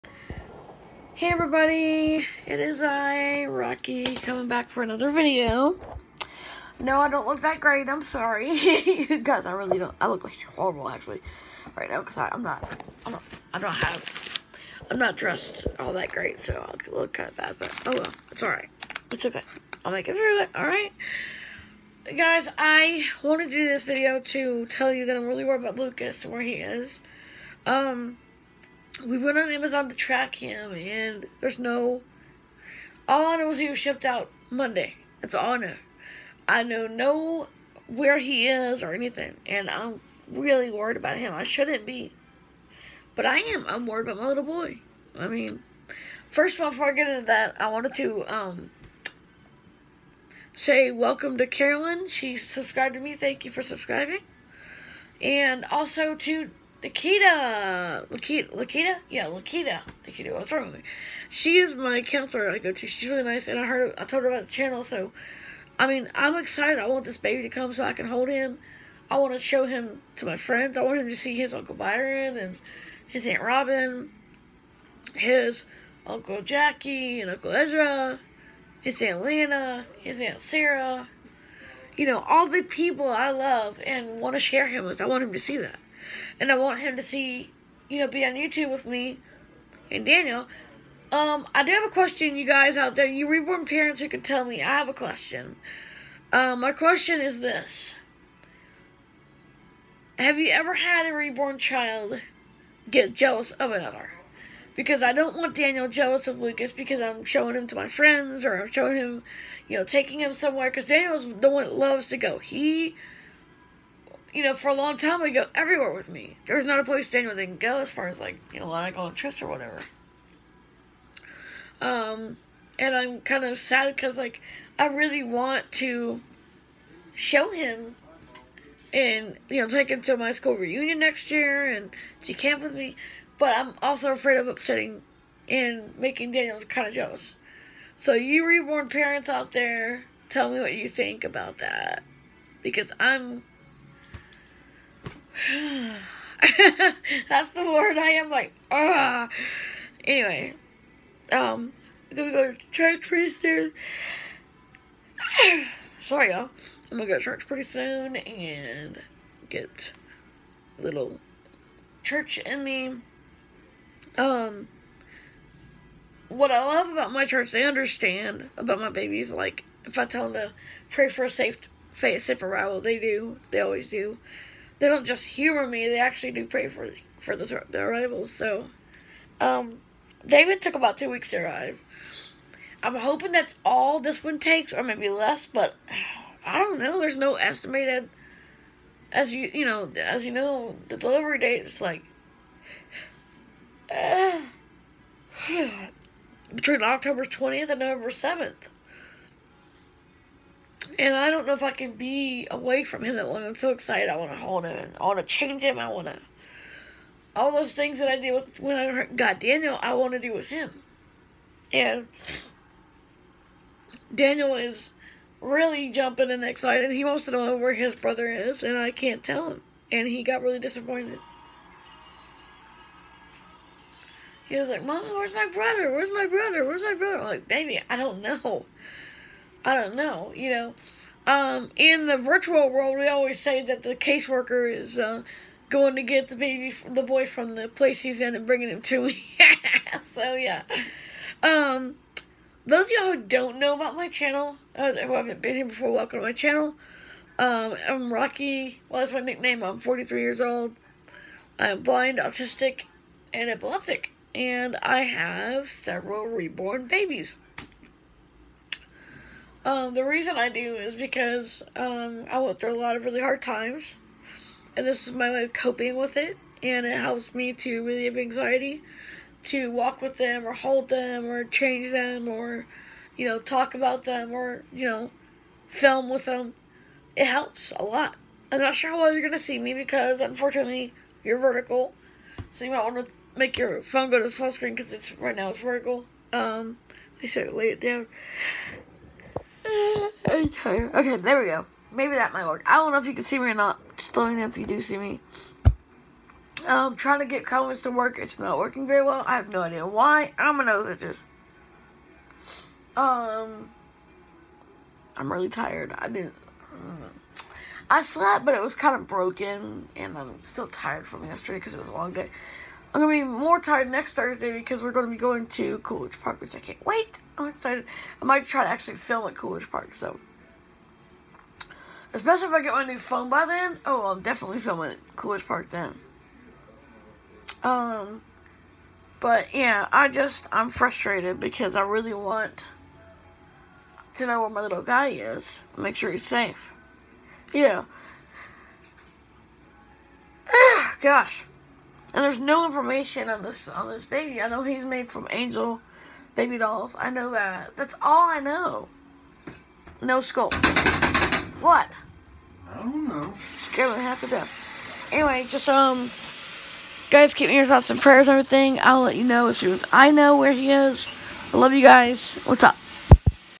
WeDon’tNo where Lucas is￼
HiEveryone well, we have no idea where are we born son Lucas is right now. Pray for me because we have no idea where he is all we know is that he was shipped on Monday that’s all we know. Anyway, pray for me love you guysHis estimated delivery date is October 20 to November 7. So God knows what alone knows what that’s gonna be. Anyways, love you guys￼￼